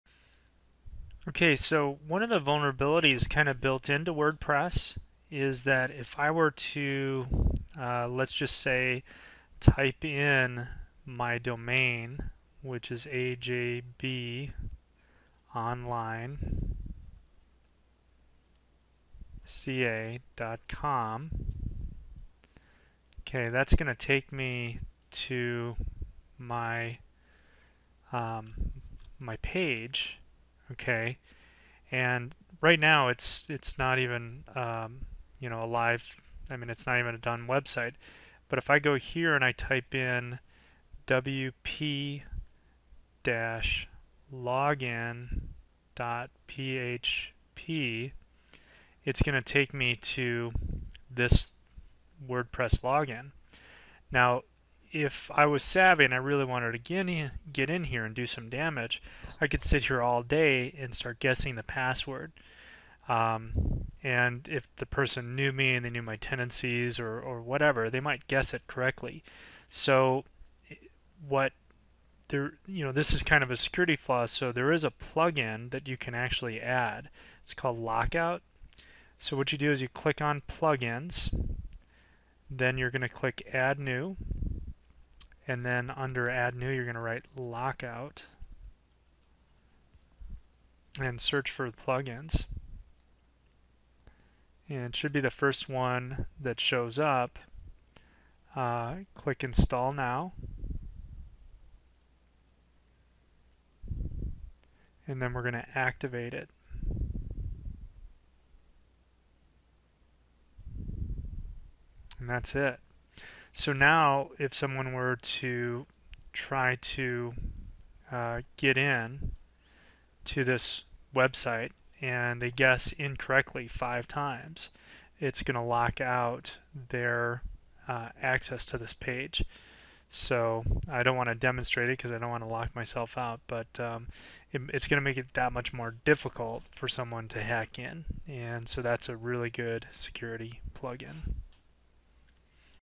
How protect your WordPress site with "Lock Out" Plugin
your site is always going to be vulnerable to hackers that can guess your password. By installing this plug-in it'll make it harder for hackers to guess your password. If the attempt to login unsuccessfully five times they are blocked from the site. Most hackers are going to move on to easier targets.